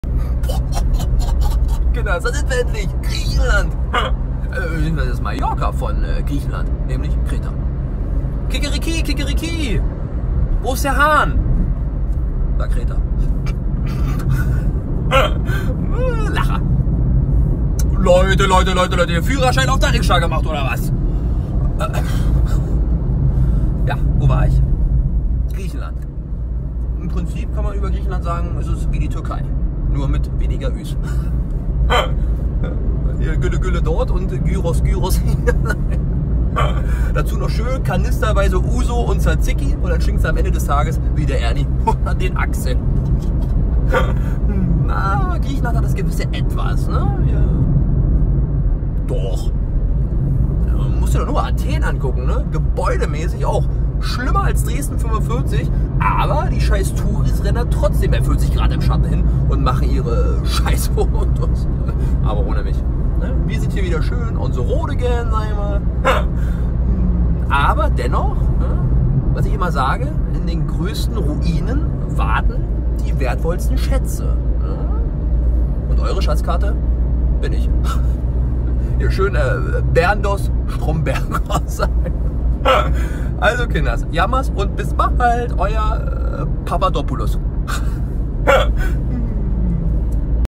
Young Stromberg #29 - Griechenland
Im Prinzip ist Griechenland ja wie die Türkei...nur mit weniger Üs! Also statt Güle Güle halt Gyros Gyros! 🍽 Viel Spaß mit den Einschätzung von eurem Reiseführer...PAPAdopoulos! 🇬🇷👨🏻‍💼
Young Stromberg - Inspiriert von der deutschen Brainpool TV GmbH-Serie "Stromberg" von Ralf Husmann.